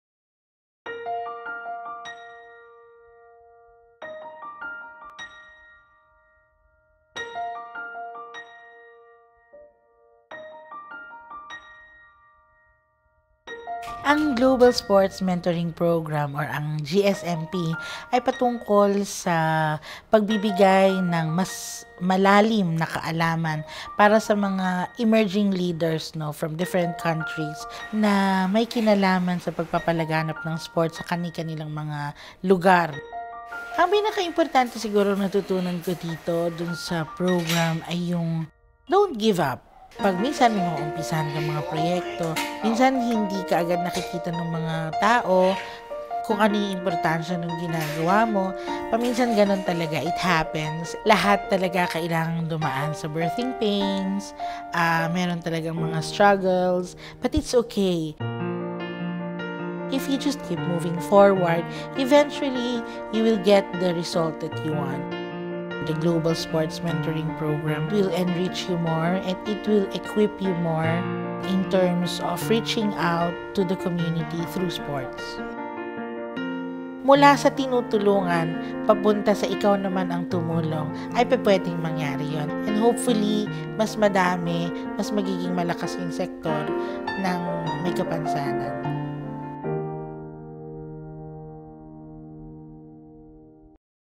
Alumna Adz Dumapong on how the Global Sports Mentoring Program...
“There are struggles, but it’s okay.  If you just keep moving forward, eventually you will get the result that you want.” - Adz Dumapong, Filipina Paralympic medalist and Global Sports Mentoring Program alumna.

This International Women’s Day, we are inspired by the story Adz Dumapong, who reaches out to communities through sports and hopes to empower persons with disabilities.